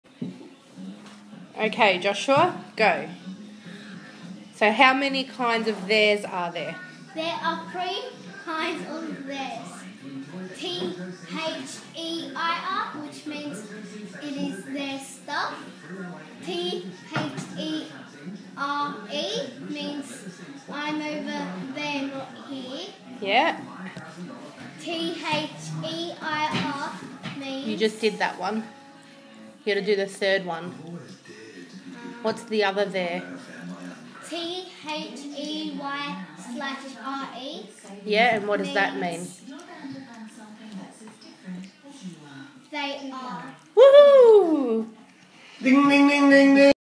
Our Asperger son, doing a better job at distinguishing the difference between Their, There & they're
He's doing better than most adults !